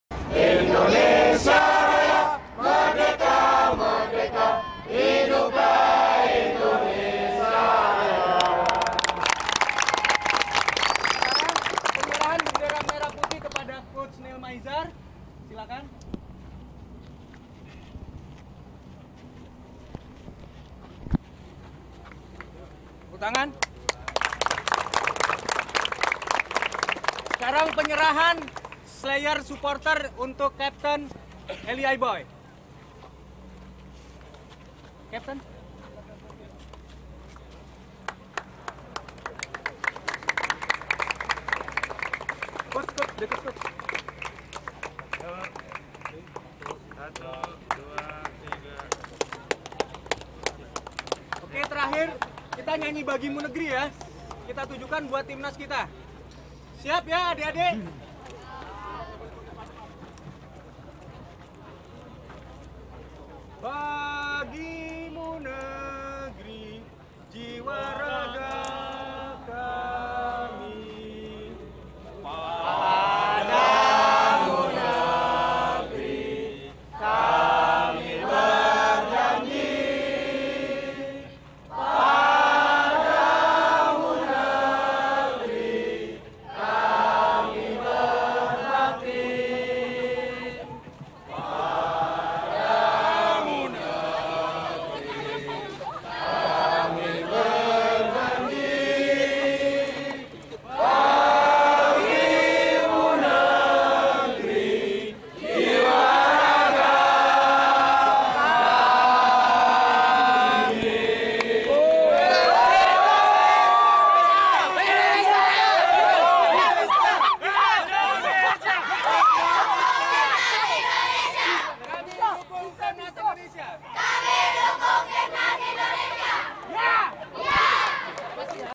Dukungan Suporter terhadap Timnas Indonesia dan Coach Nil Maizar sebelum AFF 2012
Walau sedang dilanda badai dari mafia-mafia sepakbola ( KPSI) , Isu penarikan 9 pemain timnas oleh manajemen Semen padang diakarenakan pindah ke ISL dan gerbong KPSI.Timnas indonesia terus mendapatkan dukungan dari suporter yang begitu mencintai timnas Indonesia. c